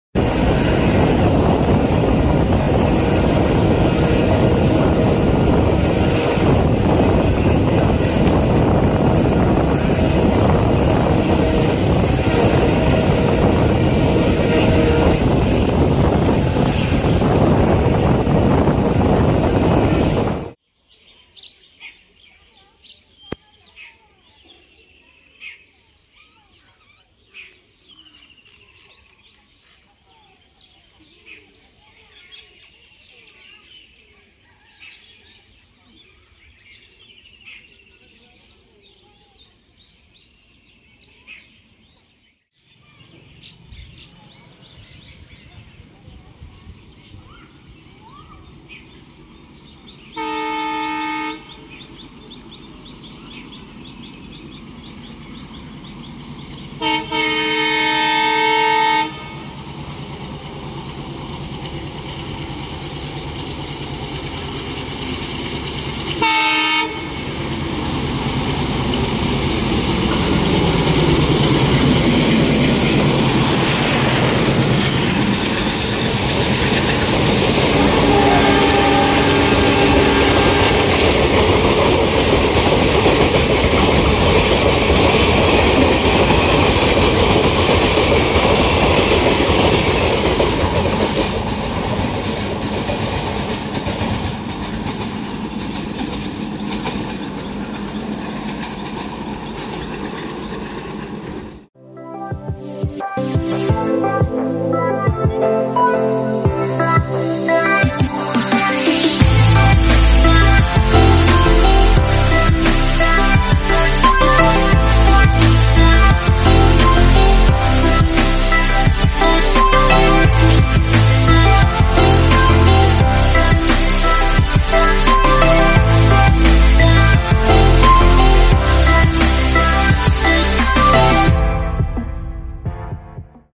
😎 DIFFERENT TYPE CLOUR OF MALDA ALCO !! IN LEAD LUMDING INTERCITY EXP. THRASHES PTLD WITH FULL MPH.
AND WELCOME TO TODAY'S NEW VIDEO

SO, IN THIS VIDEO...

"15769" UP/ ALIPURDUAR JUN. LUMDING INTERCITY EXPRESS POWERED BY DIFFERENT CLOUR MALDA WDM-3D #11475 | SKIPPING PTLD WITH FULL THROTTLE AND RUSHING TOWARDS NEXT HALT BARPETA ROAD RAILWAY STATION.

SO, EVERYONE I HOPE YOU ENJOY THE MALDA ALCO ACTION IN HIGH SPEED.

BYE ALL 👋👋